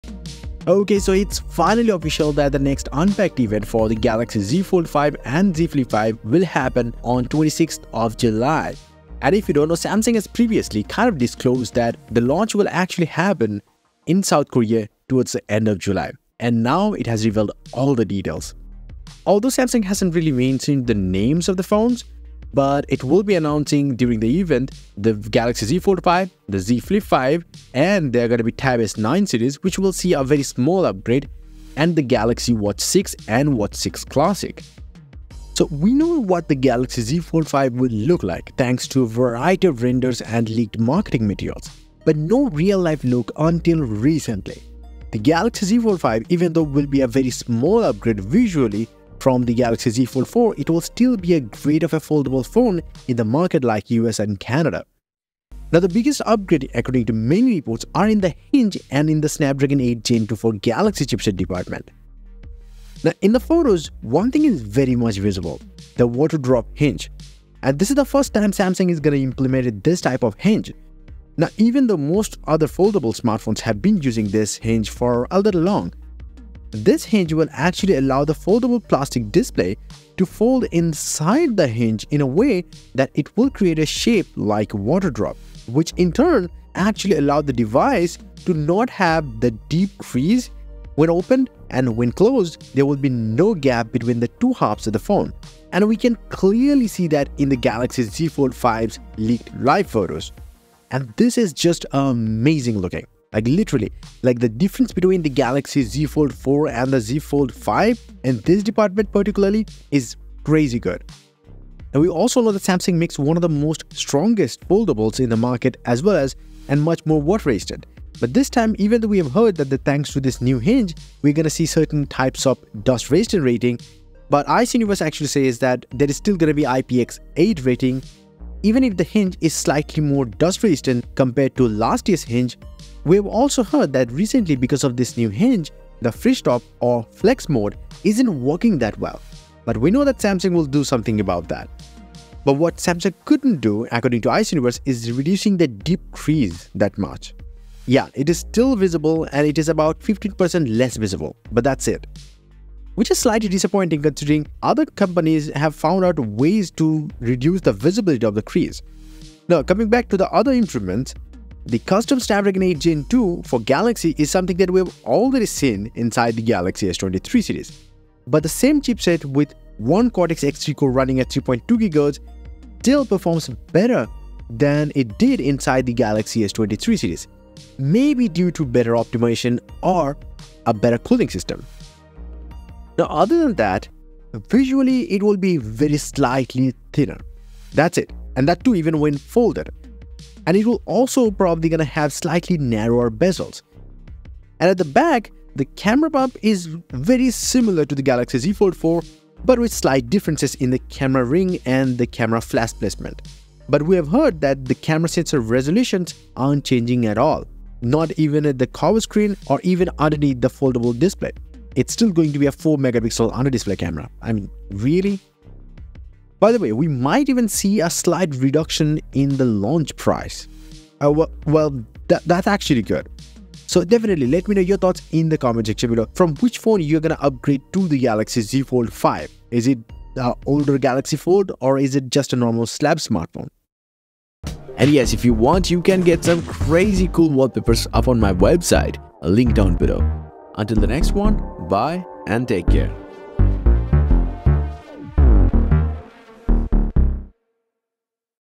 Galaxy Z Fold 5 - SURPRISE! Samsung
The Galaxy Z Fold 5 will replace the Samsung galaxy Z Fold 4 in the market. The Galaxy Z Fold 5 will have some significant specification, and hinge upgrades, & Galaxy Z Fold 5 will be powered by snapdragon 8 plus gen 2, and many more.  Today we have Everything We Know So Far about The Galaxy Z Fold 5 and first look at the official photos of the Galaxy Z Fold 5. Check Out The Episode to know more about the Galaxy Z Fold 5 specs upgrades and first look.

galaxy z fold, galaxy z fold5, samsung, Samsung Fold 5, fold 5

Music
Copyright to their original owner.